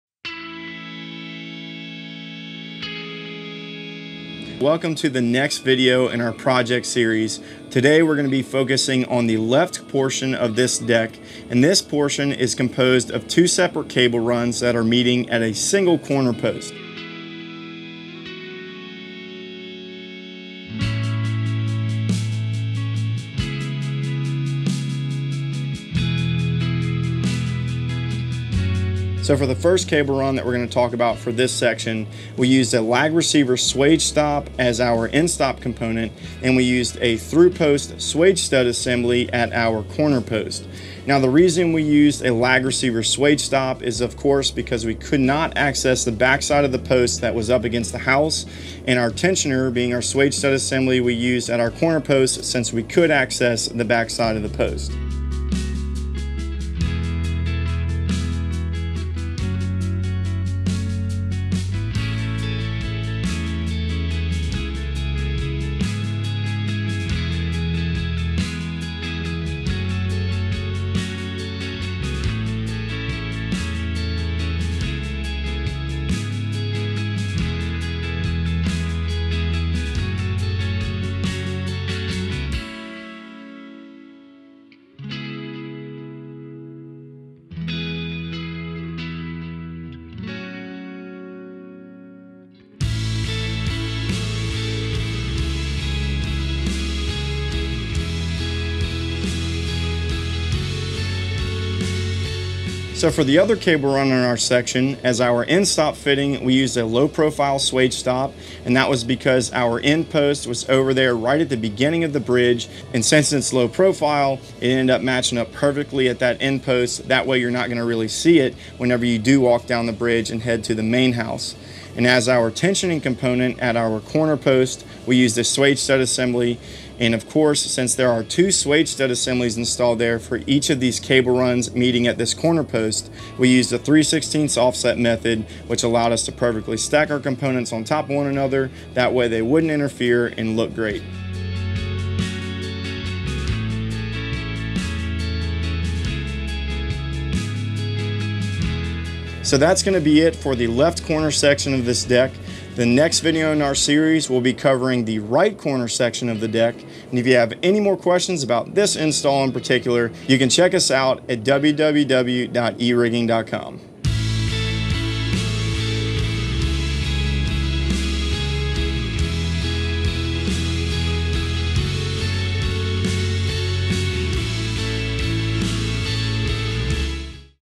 Cable Railing Project Series - 'The Mechanic' House - Part 5
Part 5! Tyler demonstrates best-use scenarios for Lag Receiver Swage Stops and Low-profile Swage Stops in the left corner section of the property used in the 2011 film, The Mechanic.